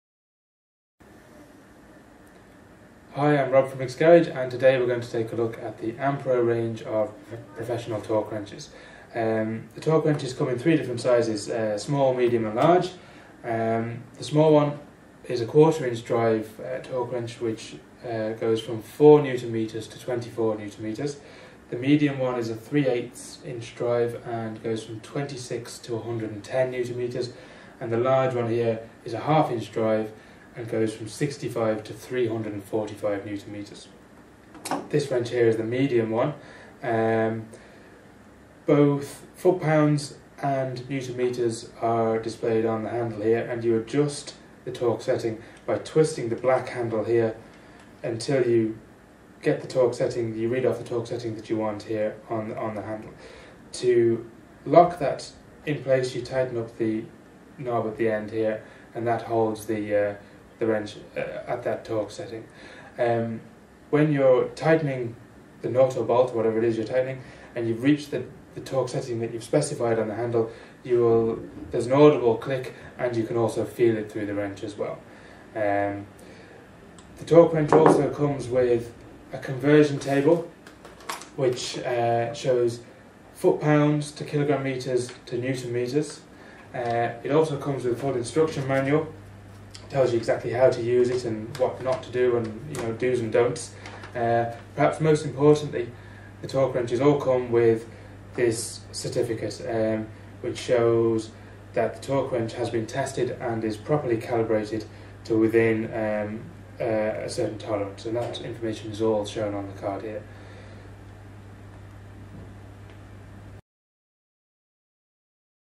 AmPro Torque Wrench
The MicksGarage.com Experts demos the AmPro Torque Wrench